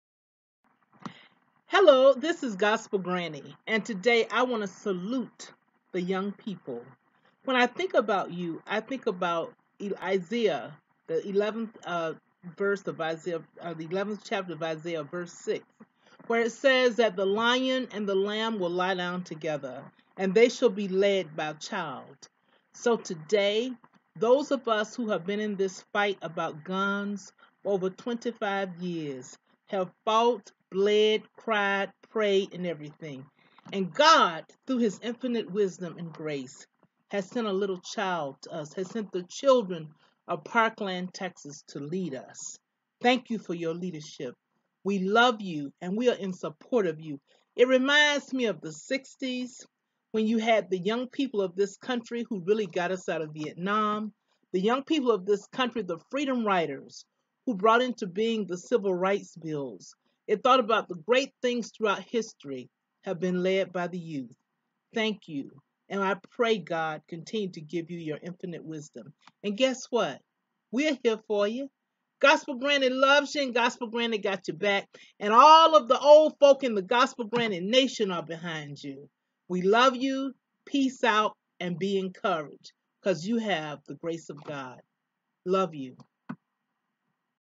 Gospel Granny Praises Parkland Isaiah 11 and 6
Gospel Granny Praises Parkland and quotes Isaiah 11:6
, , ,  And A Little Child Shall Lead Them.